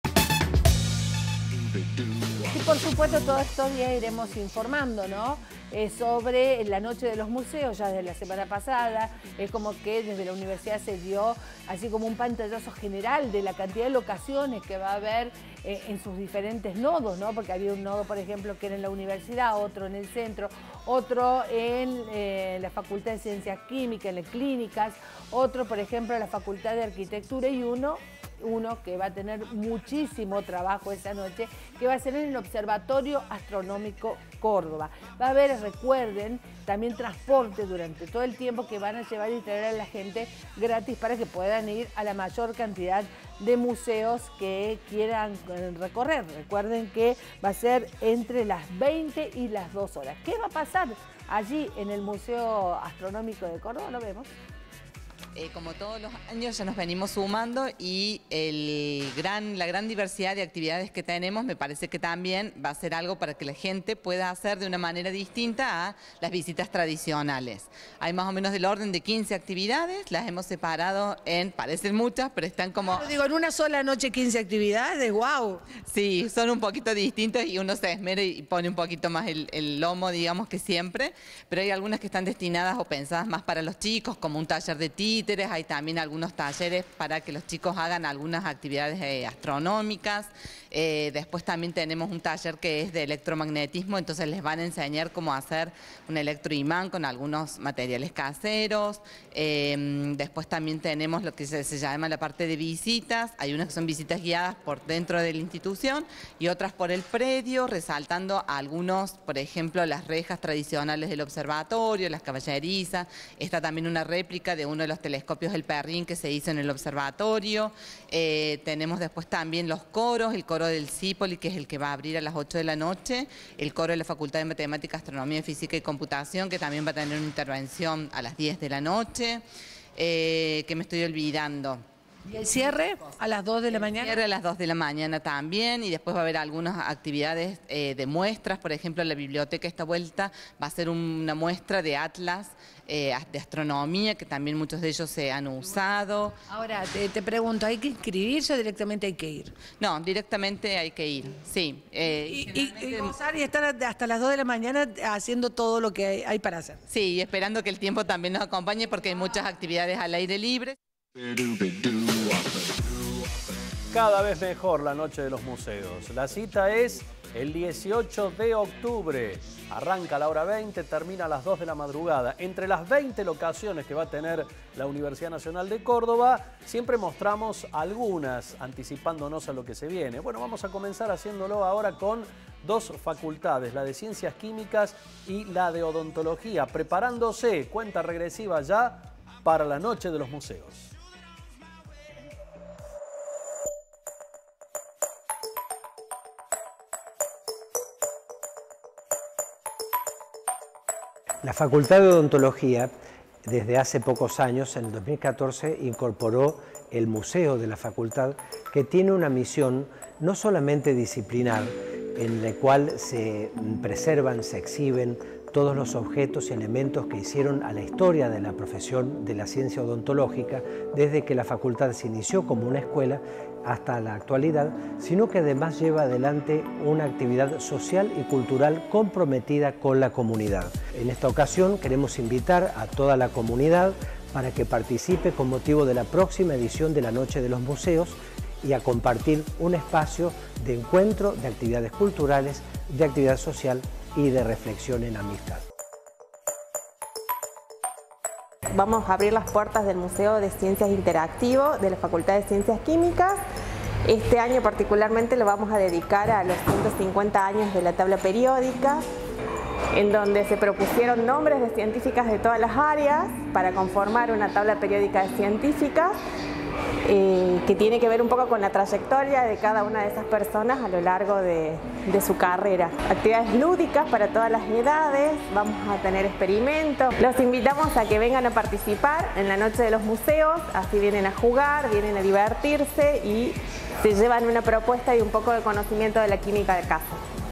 Se viene la Noche de los Museos 2019
El próximo 18 de octubre, de 20 a 02 horas, museos y espacios culturales de Córdoba abrirán sus puertas proponiendo recorridos libres y visitas guiadas a sus colecciones, como también, charlas, espectáculos musicales, teatrales, audiovisuales, experiencias lúdicas, talleres y demostraciones científicas. La Universidad tendrá 20 locaciones distintos lugares. En Data Universitaria te mostraremos la del Observatorio Astronómico, Facultad de Odontología y Facultad de Ciencias Químicas.